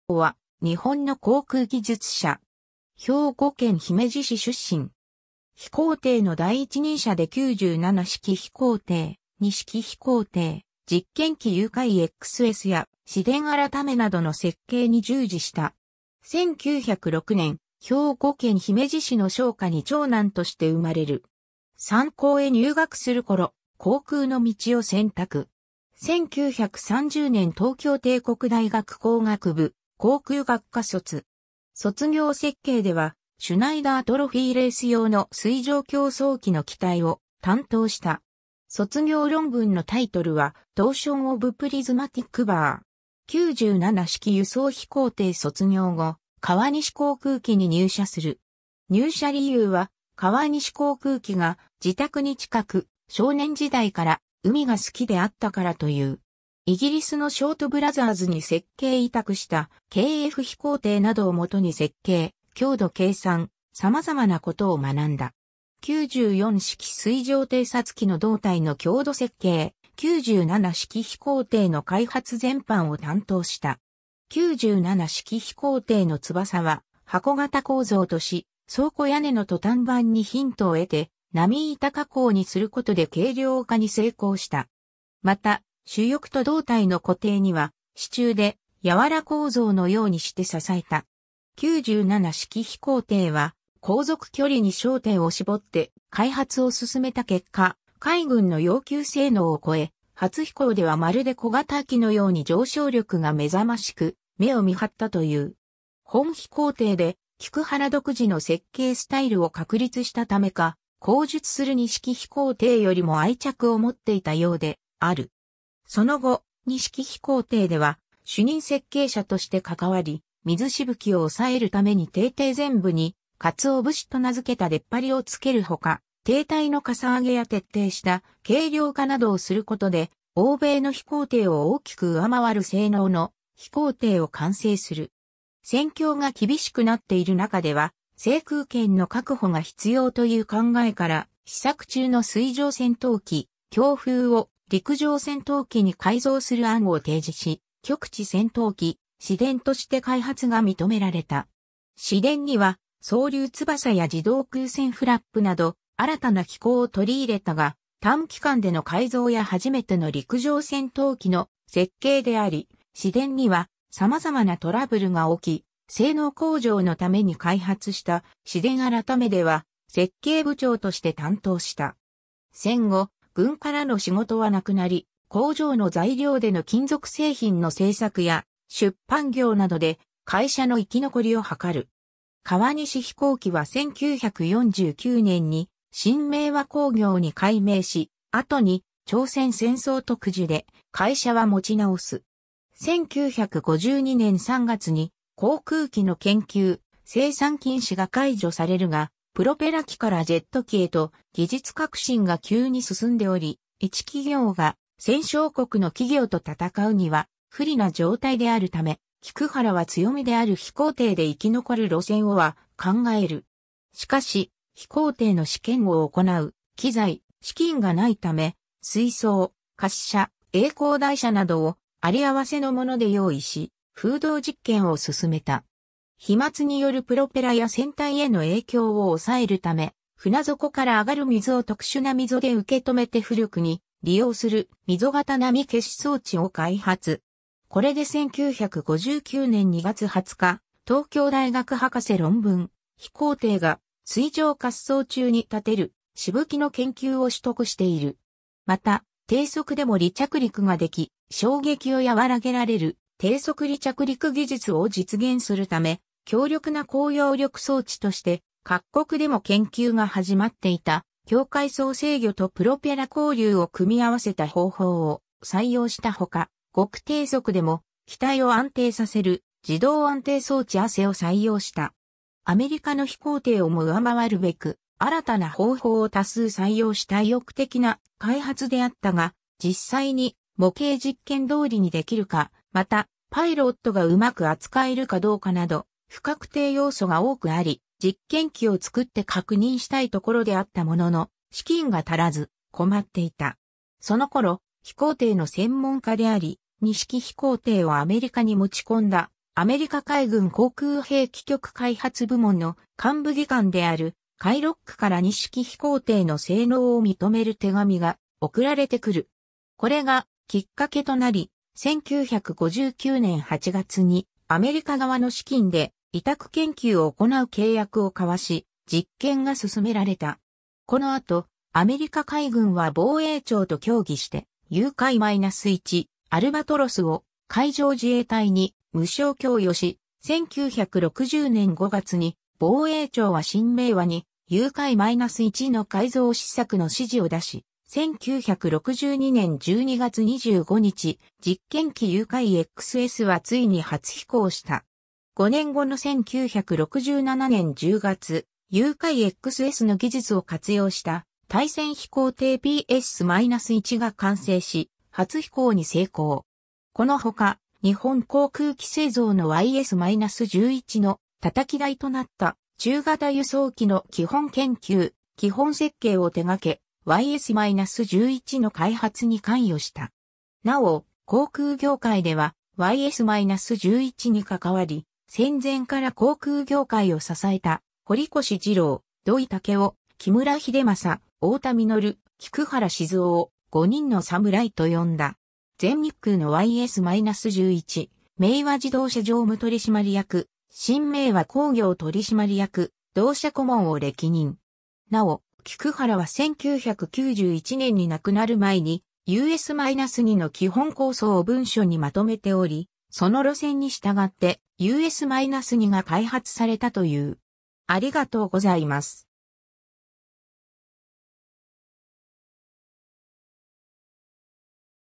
菊原静男
菊原 静男（きくはら しずお、1906年 - 1991年8月6日）は、日本の航空技術者。
兵庫県姫路市出身。
飛行艇の第一人者で九七式飛行艇、二式飛行艇、実験機UF-XSや紫電改などの設計に従事した。
 1906年、兵庫県姫路市の商家に長男として生まれる。
三高へ入学する頃、航空の道を選択。
1930年（昭和5年）東京帝国大学工学部航空学科卒。
卒業設計では、シュナイダー・トロフィー・レース用の水上競争機の機体を担当した。
卒業論文のタイトルは「Torsion of Prismatic Bar．」。
 九七式輸送飛行艇 卒業後、川西航空機（現新明和工業）に入社する。
入社理由は、川西航空機が自宅に近く、少年時代から海が好きであったからという。
イギリスのショート・ブラザーズに設計委託した「KF飛行艇」などを基に設計、強度計算、様々なことを学んだ。
九四式水上偵察機の胴体の強度設計、九七式飛行艇の開発全般（設計主任補佐）を担当した。
九七式飛行艇の翼は箱型構造とし、倉庫屋根のトタン板にヒントを得て、波板加工にすることで軽量化に成功した。
また、主翼と胴体の固定には、支柱で柔構造のようにして支えた。
九七式飛行艇は航続距離に焦点を絞って開発を進めた結果、海軍の要求性能を越え、初飛行ではまるで小型機のように上昇力が目覚ましく目を見張ったという。
本飛行艇で菊原独自の設計スタイルを確立したためか、後述する二式飛行艇よりも愛着を持っていたようである。
その後、二式飛行艇では主任設計者として関わり、水飛沫を抑えるために艇底前部に「かつお節」と名付けた出っ張りをつける他、艇体の嵩上げや徹底した軽量化などをすることで、欧米の飛行艇を大きく上回る性能の飛行艇を完成する。
 戦況が厳しくなっている中では制空権の確保が必要という考えから、試作中の水上戦闘機「強風」を陸上戦闘機に改造する案を提示し、局地戦闘機「紫電」として開発が認められた。
紫電には層流翼や自動空戦フラップなど、新たな機構を採り入れたが、短期間での改造や初めての陸上戦闘機の設計であり、紫電には様々なトラブルが起き、性能向上のために開発した紫電改では、設計部長として担当した。
 戦後、軍からの仕事は無くなり、工場の材料での金属製品の製作や出版業などで会社の生き残りを図る。
川西飛行機は1949年に新明和興業に改名し、後に朝鮮戦争特需で会社は持ち直す。
1952年3月に「航空機の研究・生産禁止」が解除されるが、プロペラ機からジェット機へと技術革新が急に進んでおり、一企業が戦勝国の企業と戦うには不利な状態であるため、菊原は強みである飛行艇で生き残る路線をは考える。
しかし、飛行艇の試験を行う器材・資金が無いため、水槽・滑車・曳航台車などを有り合わせの物で用意し、風洞実験を進めた。
飛沫によるプロペラや船体への影響を抑えるため、船底から上がる水を特殊な溝で受け止めて浮力に利用する「溝型波消し装置」を開発。
これで1959年2月20日、東京大学博士論文「飛行艇が水上滑走中にたてる飛沫の研究」を取得している。
また、低速でも離着陸ができ、衝撃を和らげられる「低速離着陸（STOL）技術」を実現するため、強力な高揚力装置として、各国でも研究が始まっていた境界層制御（BLC）とプロペラ後流を組み合わせた方法を採用したほか、極低速でも機体を安定させる「自動安定装置ASE」を採用した。
アメリカの飛行艇をも上回るべく、新たな方法を多数採用した意欲的な開発であったが、実際に模型実験通りにできるか、また、パイロットがうまく扱えるかどうかなど、不確定要素が多くあり、実験機を作って確認したいところであったものの、資金が足らず、困っていた。
その頃、飛行艇の専門家であり、二式飛行艇をアメリカに持ち込んだ、アメリカ海軍航空兵器局開発部門の幹部技官であるF・ロックから二式飛行艇の性能を認める手紙が送られてくる。
これがきっかけとなり、1959年8月にアメリカ側の資金で委託研究を行う契約を交わし、実験が進められた。
この後、アメリカ海軍は防衛庁と協議して、UF-1「アルバトロス」を海上自衛隊に無償供与し、1960年5月に防衛庁は新明和にUF-1の改造試作の指示を出し、1962年12月25日、実験機UF-XSはついに初飛行した。
5年後の1967年10月、UF-XSの技術を活用した対潜飛行艇PS-1が完成し、初飛行に成功。
この他、日本航空機製造のYS-11の叩き台となった...